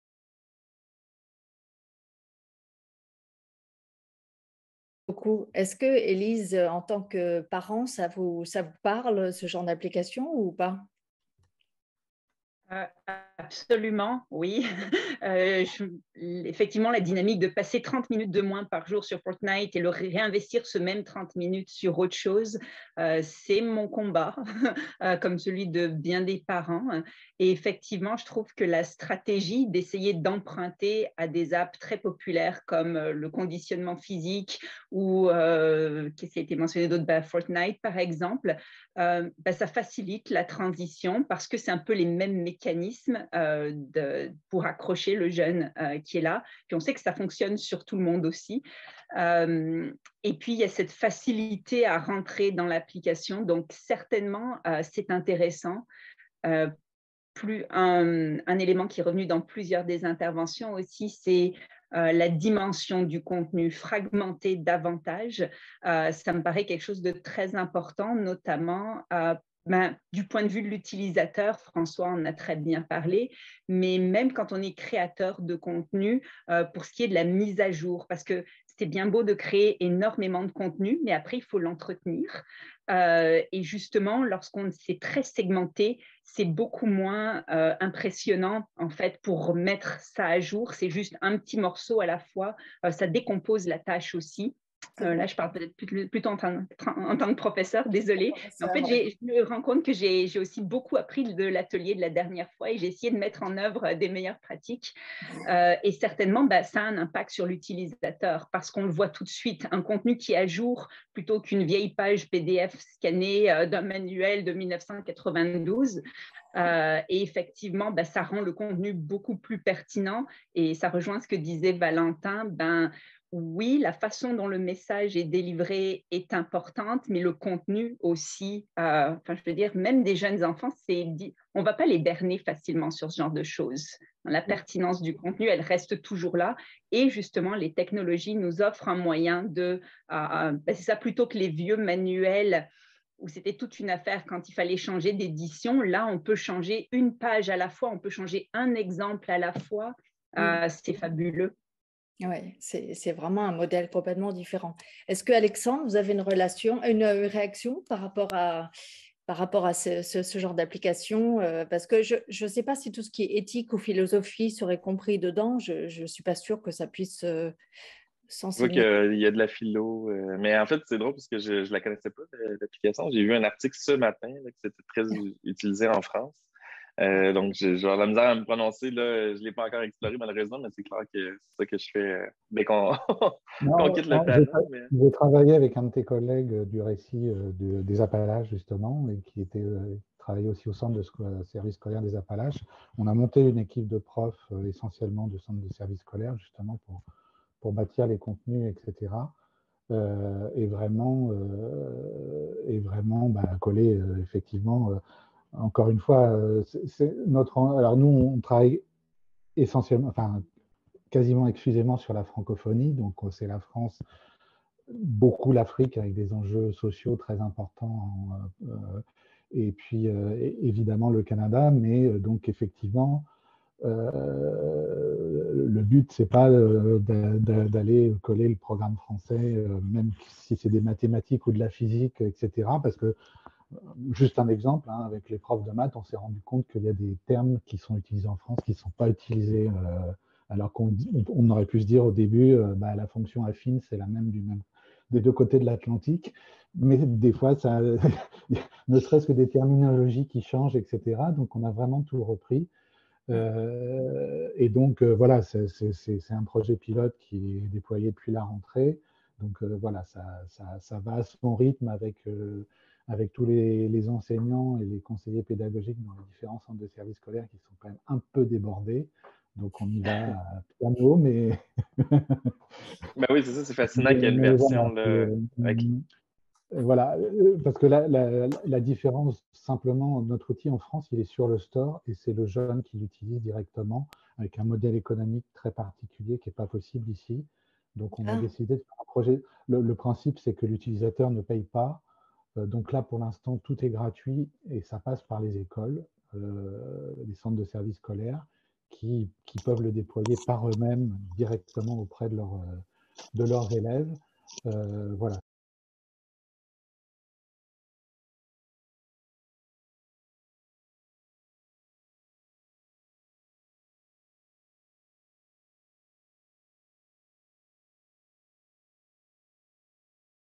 VI - Ecole et Numérique : Après 2 ans de pandémie - Janv. 22 - Partie VI
Retrouvez un extrait de la discussion citoyenne de Janvier 2022 avec 4 experts franco-canadiens sur le numérique à l'école après 2 ans de pandémie de Covid.
Partie 6 : Enseigner des notions via une application comme Nomad School